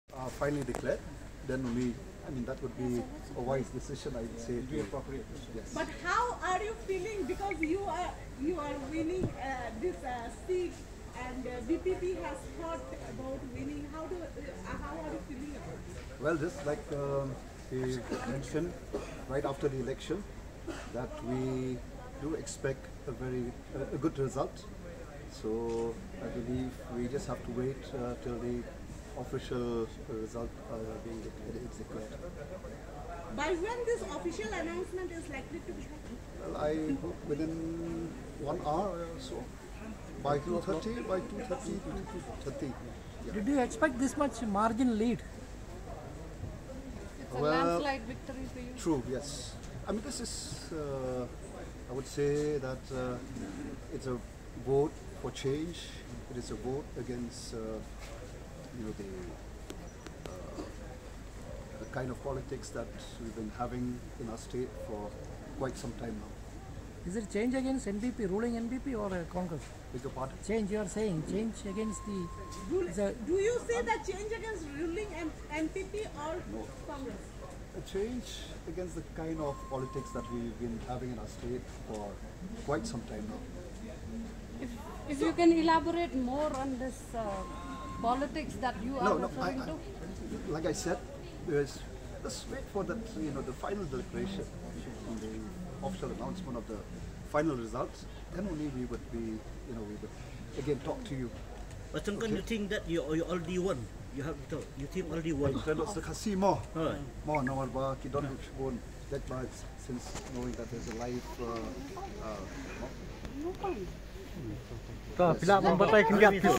VPP candidate from Shillong LS seat speaks to media after taking a massive margin on June 4
Video from rajib chowdhury